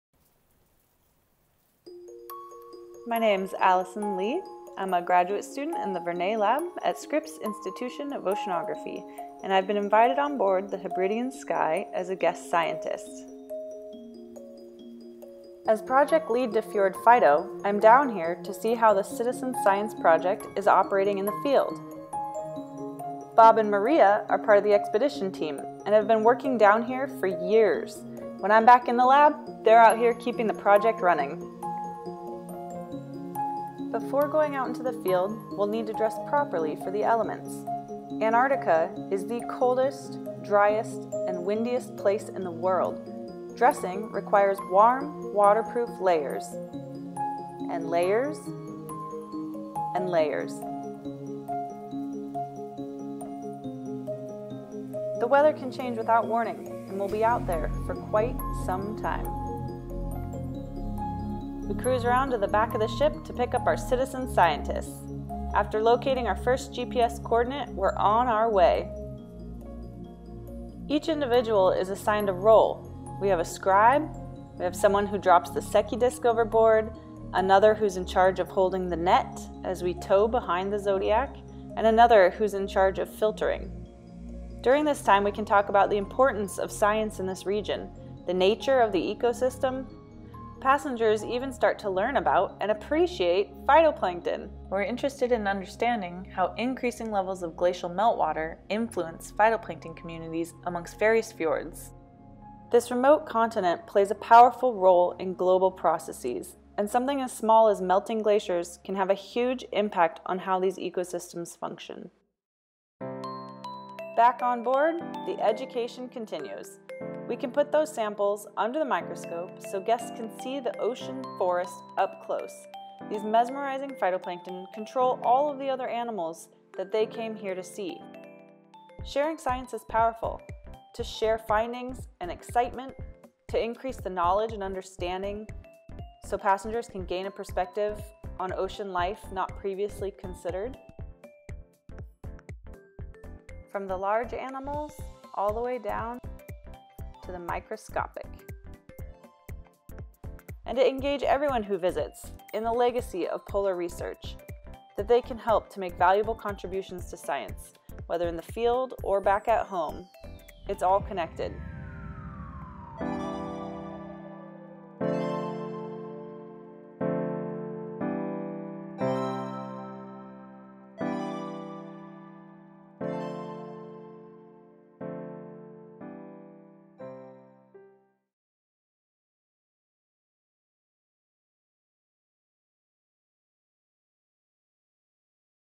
Allison Cusick as Guest Scientist with A21 2017
Allison Cusick was invited as a Guest Scientist on board the MV Hebridean Sky with the Tour Operator Antarctica21 during December 2017. for more!